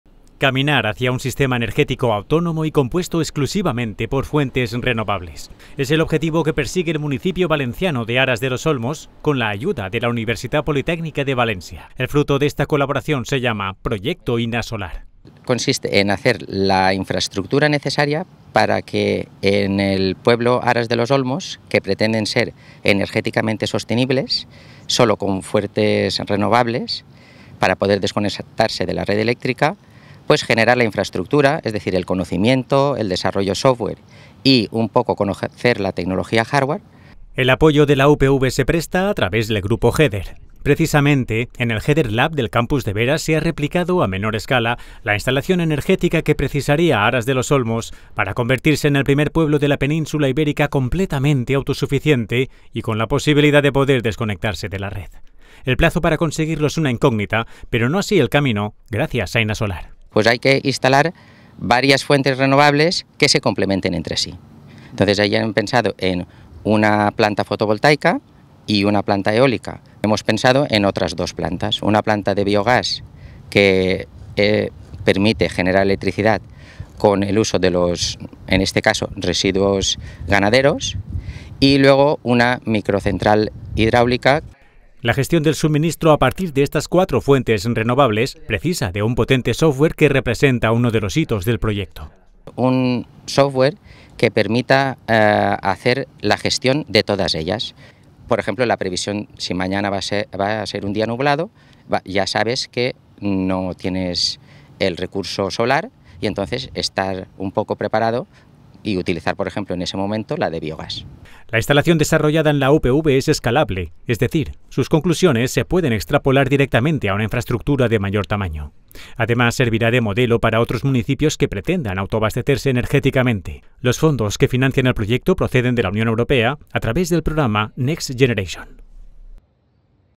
Proyecto INASOLAR - Noticia @UPVTV; 05-09-2024
La UPV impulsa la autosuficiencia energética del municipio de Aras de los Olmos a través de un proyecto sostenible y a escala que, además, sirve de modelo para otras urbes.